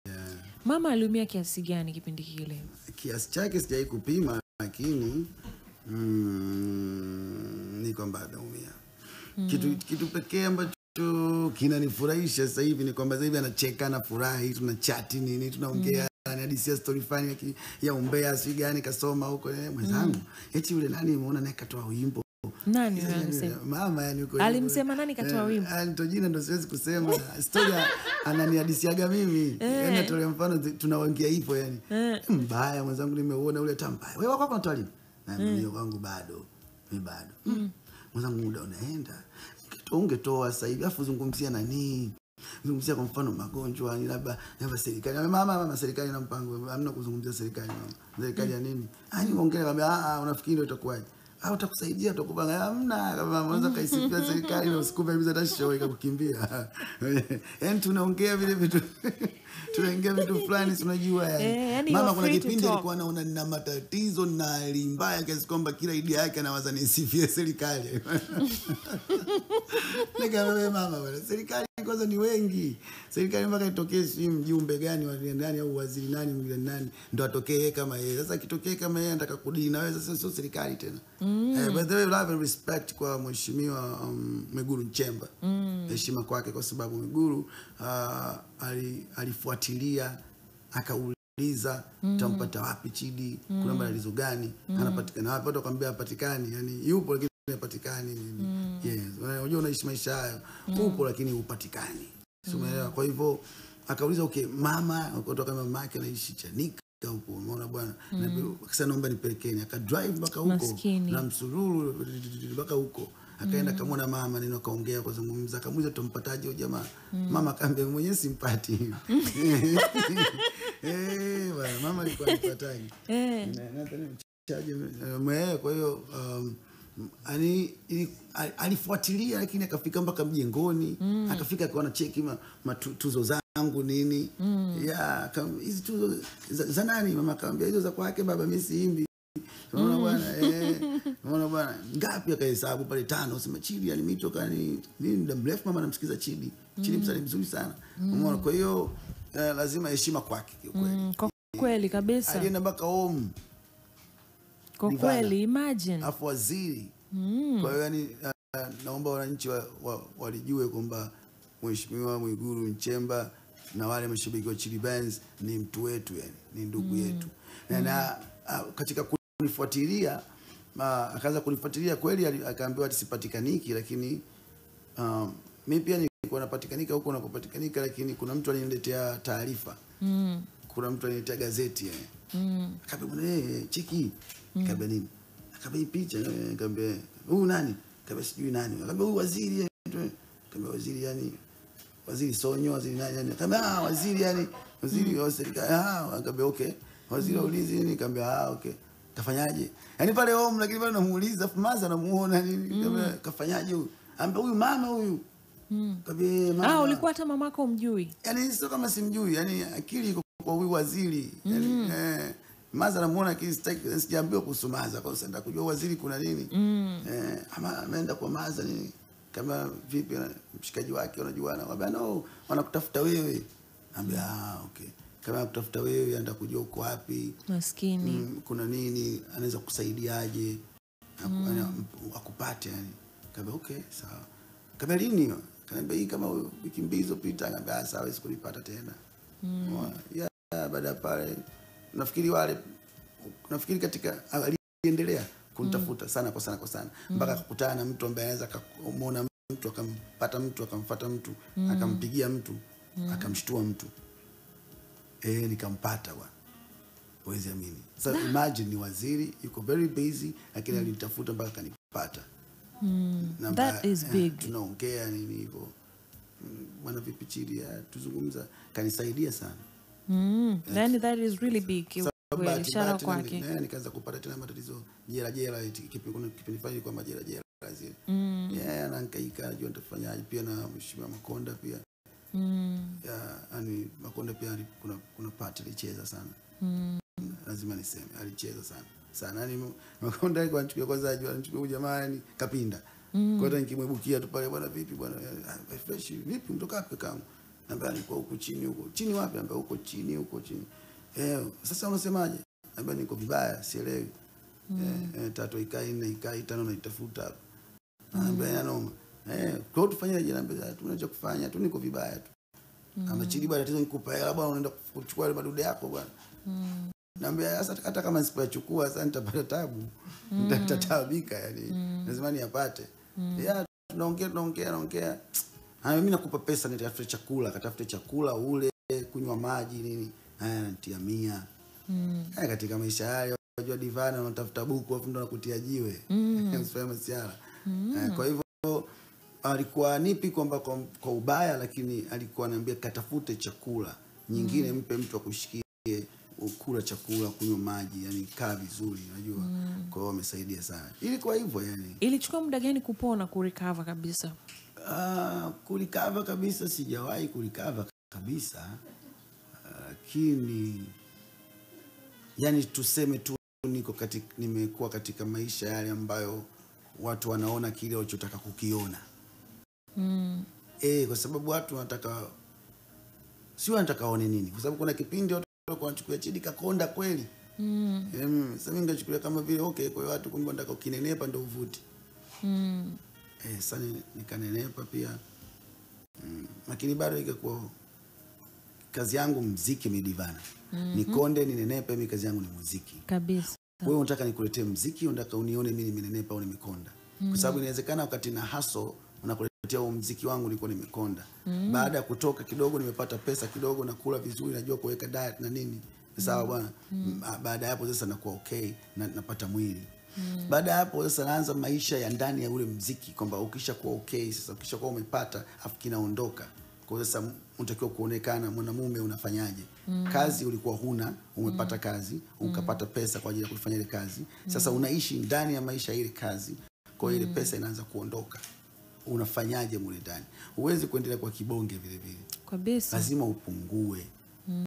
"SITAMSAHAU WAZIRI MWIGULU NCHEMBA, ALINISAIDIA SANA" - CHIDI BENZ
88.9 DAR/ZNZ/PWANI 📻 | 104.1 DODOMA 📻 | 94.5 ARUSHA 📻 | 97.3 MBEYA 📻 | 94.9 MWANZA 📻 | SHINYANGA 106.3 📻 | KIGOMA 101.7 📻